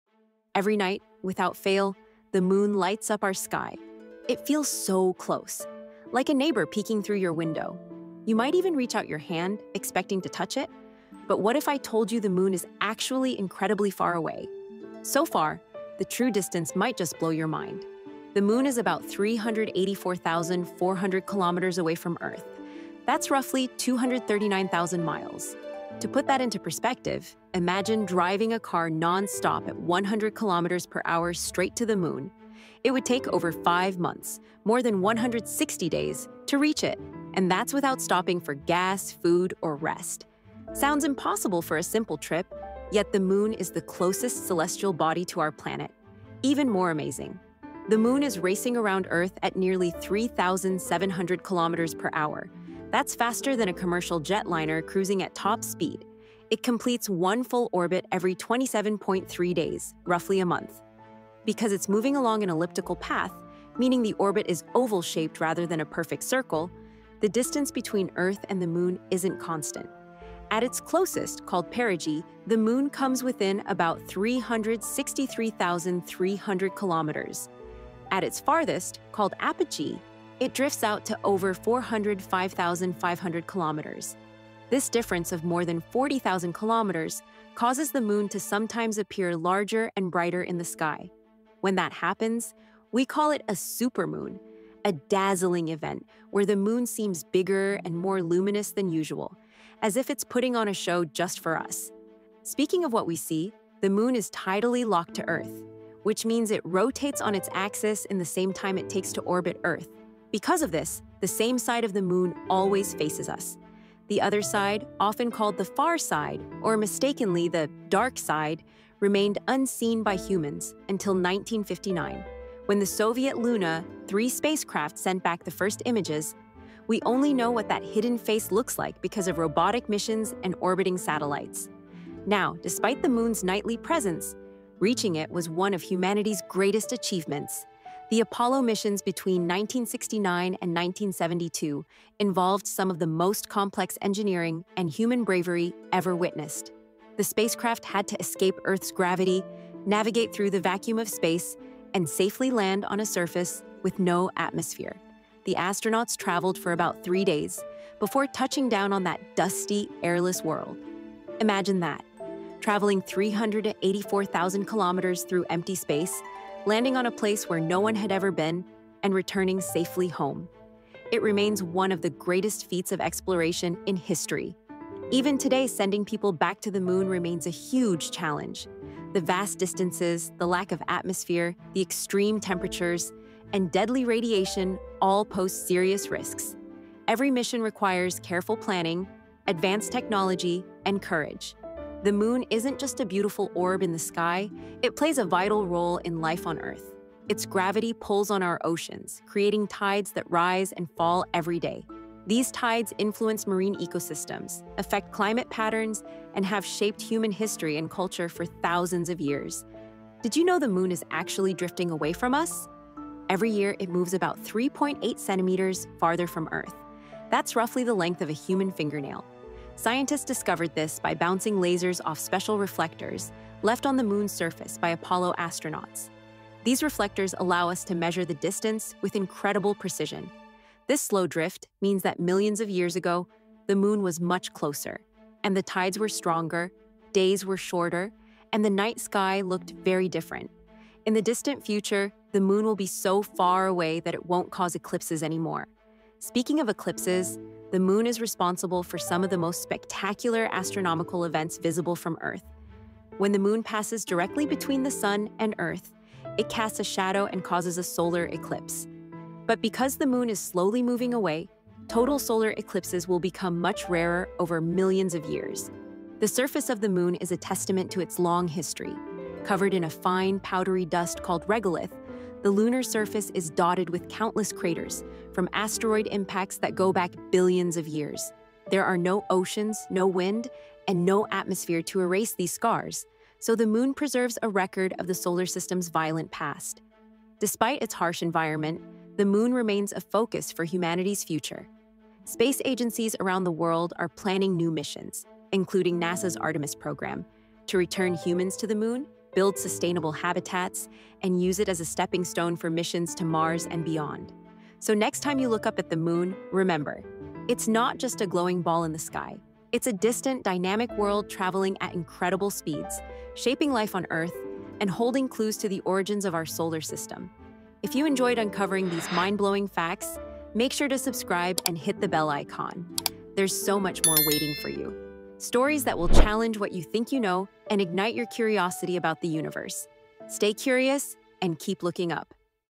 Beyond Imagination: How Far the Moon Really Is – Part 2
Think you know how far the Moon is? Think again. In Part 2 of our lunar distance series, we dive deeper into the mind-blowing scale between Earth and the Moon — with visuals that challenge everything you thought you knew. From real-world comparisons to space travel context, this episode will leave you stunned by just how far our closest neighbor truly is.

🚀 Perfect for space lovers, curious minds, and anyone fascinated by the mysteries of our universe.